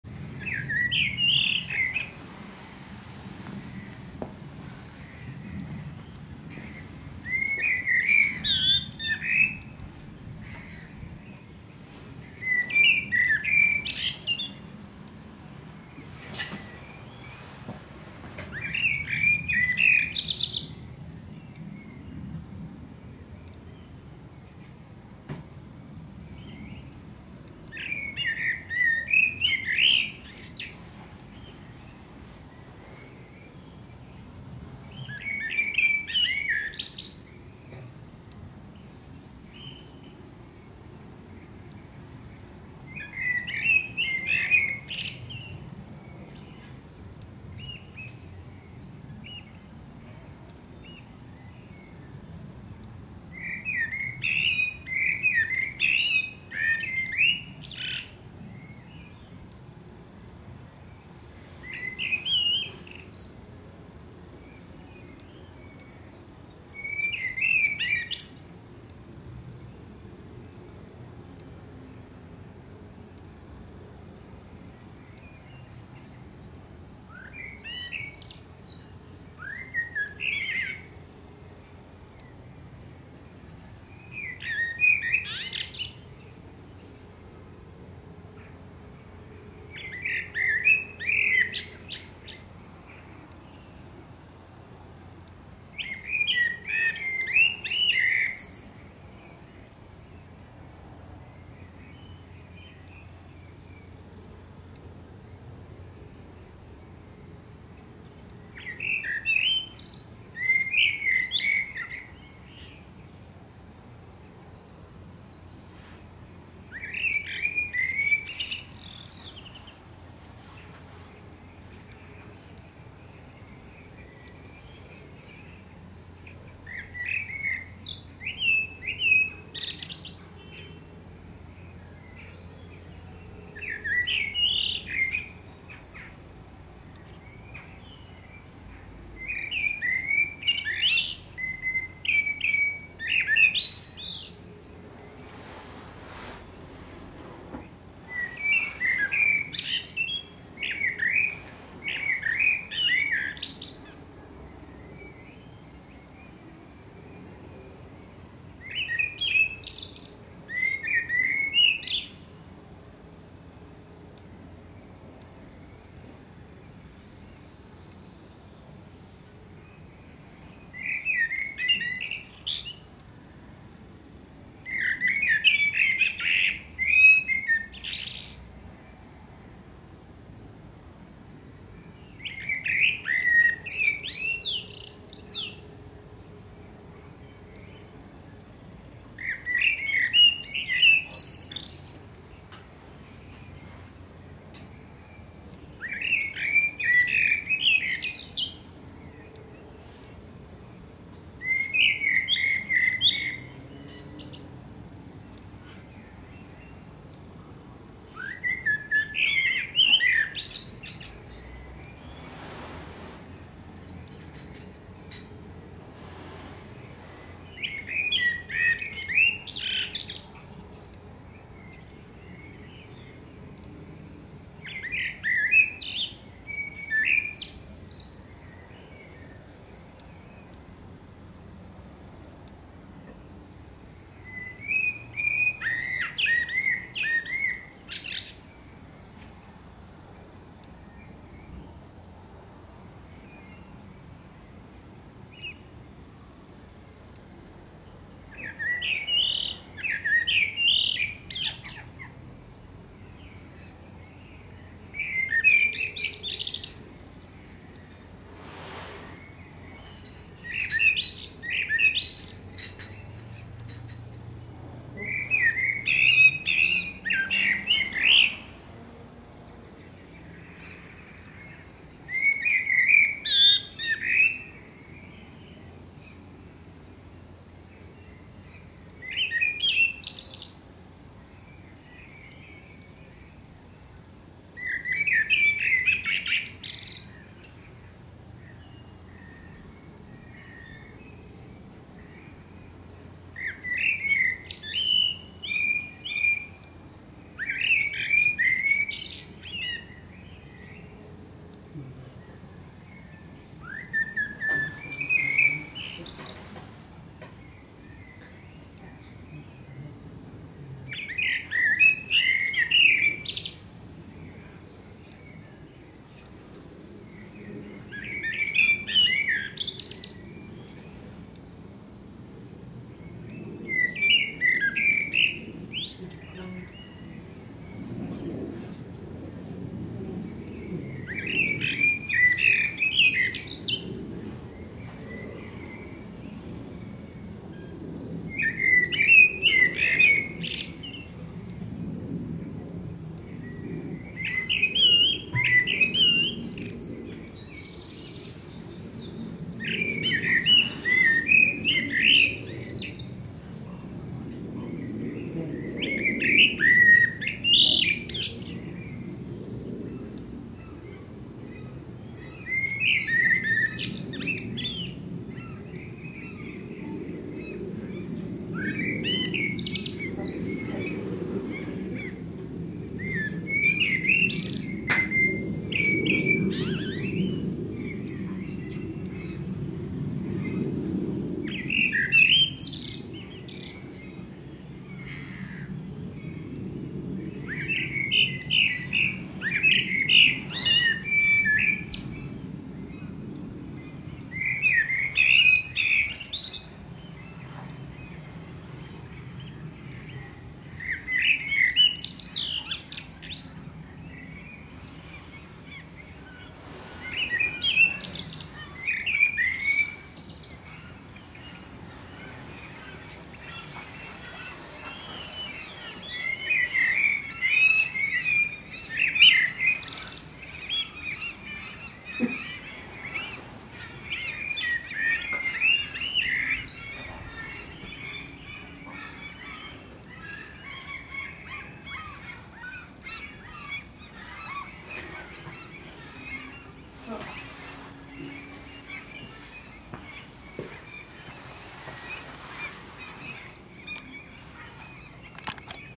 Singing Eurasian Blackbird Birds of the Netherlands
Sunset in Holland
Singing Turdus merula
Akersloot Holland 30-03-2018